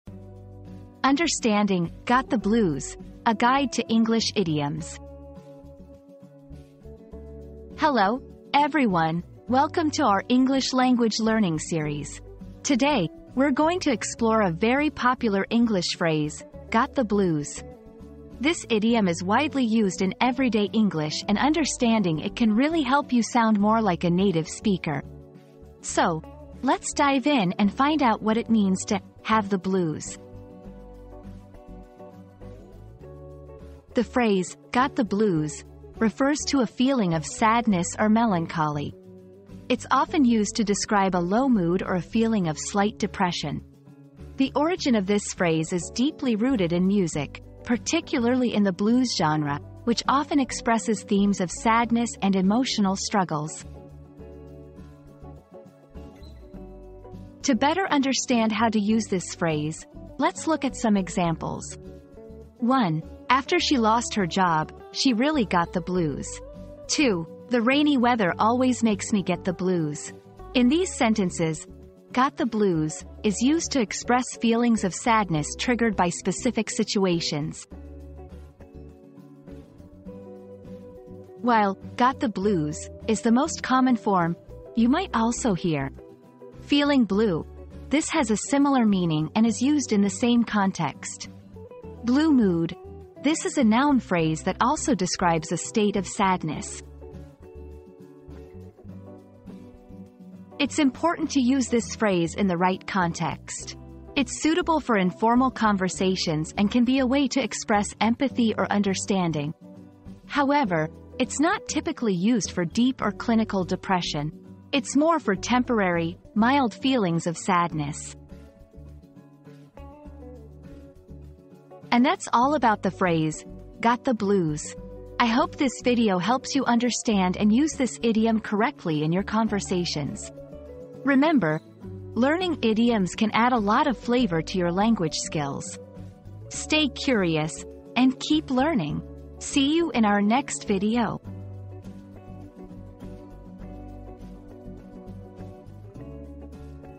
Understanding "Got the Blues": A Guide to English Idioms
00:00 • Introduction - Understanding "Got the Blues": A Guide to English Idioms
00:33 • What Does "Got the Blues" Mean?
00:56 • Examples of "Got the Blues" in Sentences
01:20 • Variations of the Phrase
01:40 • Using "Got the Blues" Appropriately